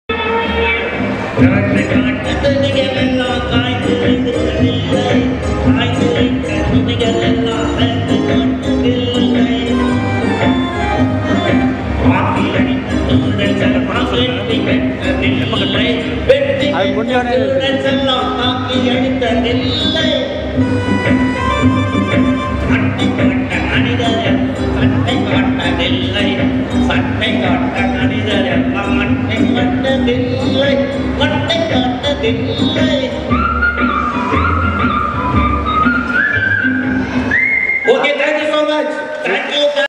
Ventriloquist Shanthakumar of BEST EVENTS CHENNAI in Kamarajar Arangam!
Ventriloquist Shanthakumar performs Ventriloquism and sings a song through Johnny in Kamarajar Arangam for M/s Abi Estate!  Actress Archana was the compere!